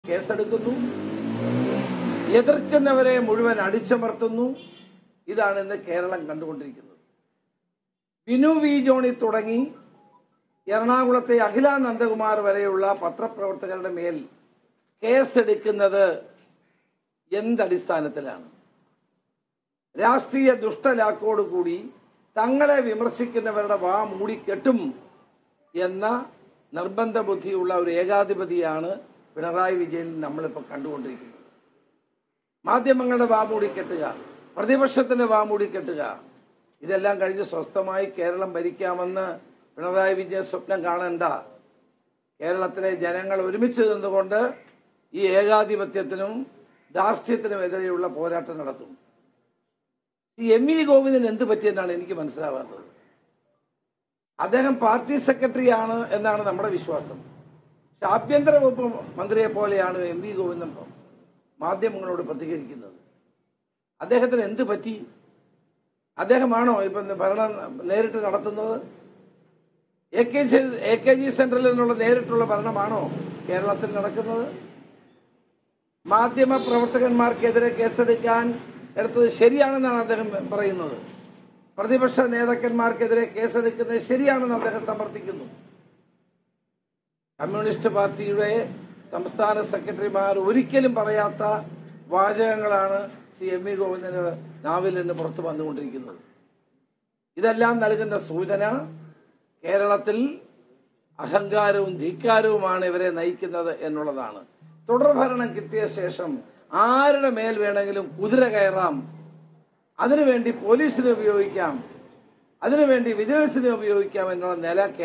AKG സെന്ററില്‍ നിന്ന് നേരിട്ടുള്ള ഭരണമാണോ കേരളത്തില്‍ നടക്കുന്നത്; രമേശ് ചെന്നിത്തല
About COA